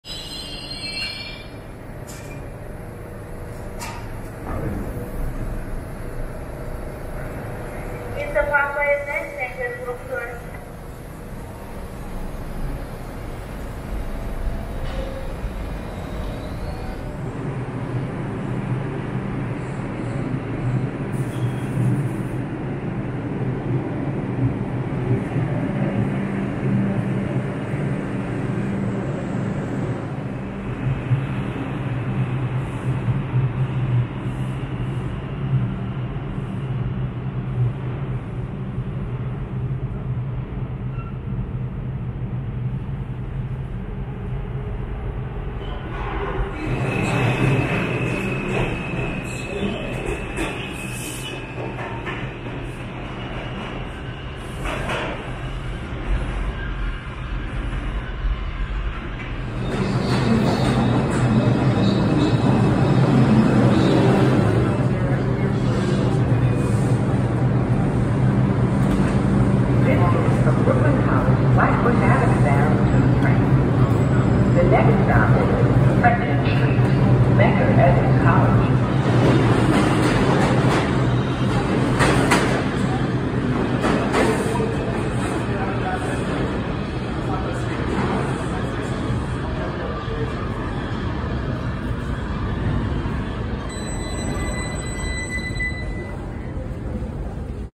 Diseased NYC Subway Tunnel Rats
Observing plain clothes NYPD cops making several unlawful stops.

This is their bread & butter. They aren’t the “Good Guys.”

They are racial profiling pigs.